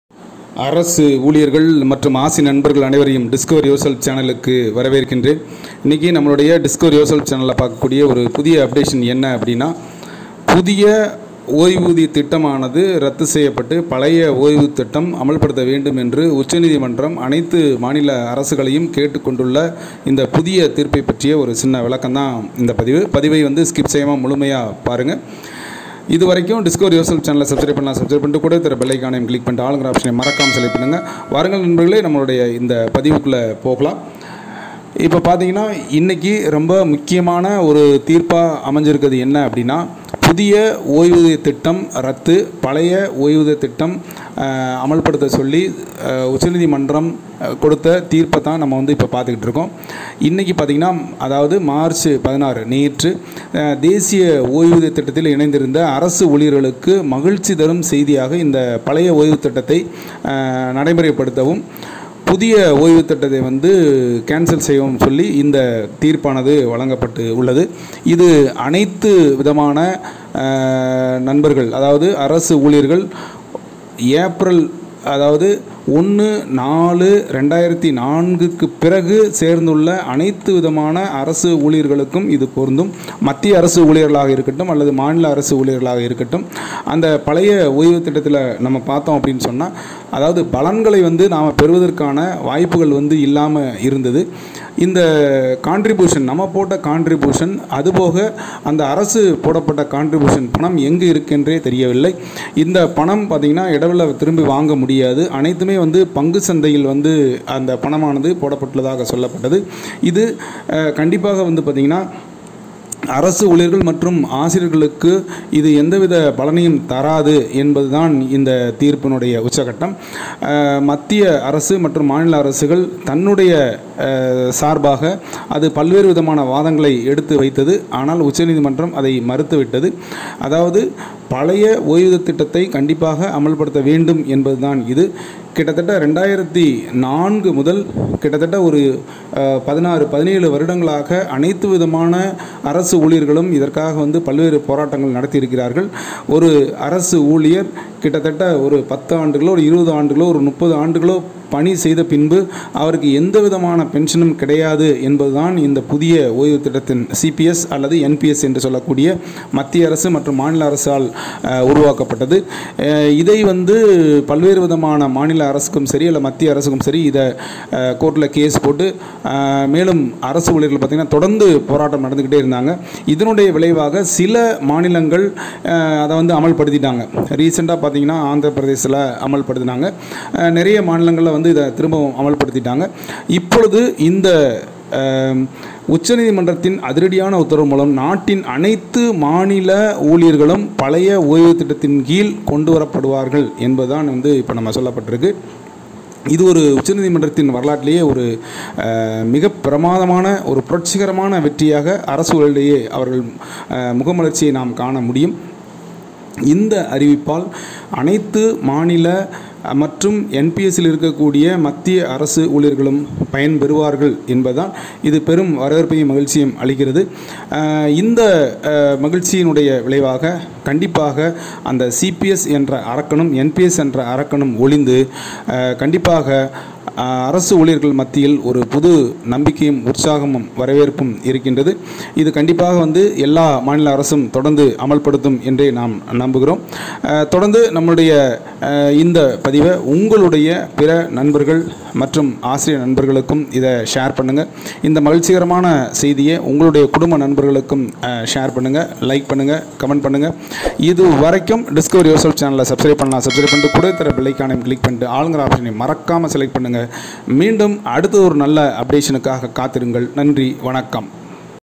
உச்சநீதிமன்றம் அதிரடி தீர்ப்பு அரசு ஊழியர்களுக்கு பழைய பென்சன் முறை
DISCOVER YOURSELF